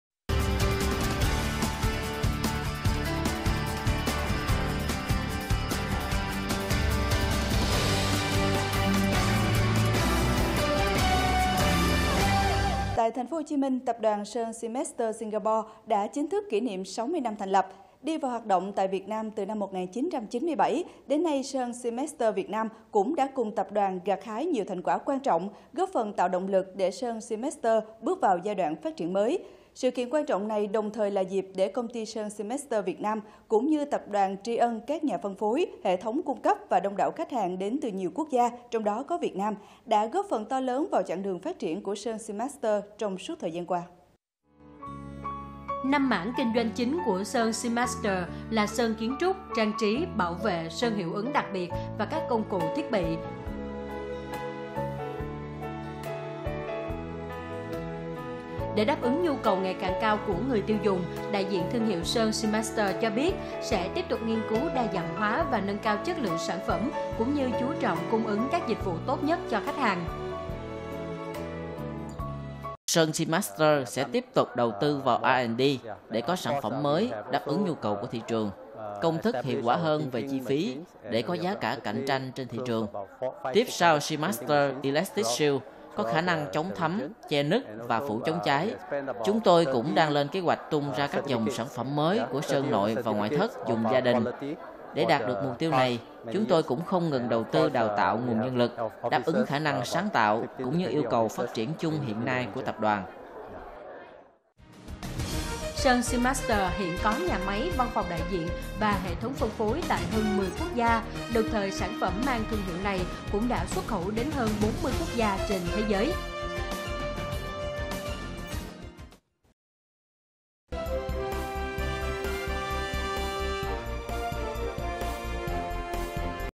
60 YEARS ANNIVERSARY OF SEAMASTER PAINT GROUP IN VIETNAM
Lễ kỷ niệm 60 năm thành lập của tập đoàn Sơn Seamaster tại Việt Nam
Buổi lễ diễn ra long trọng và thành công tốt đẹp
Với sự tham gia của hơn 300 khách hàng và giới truyền thông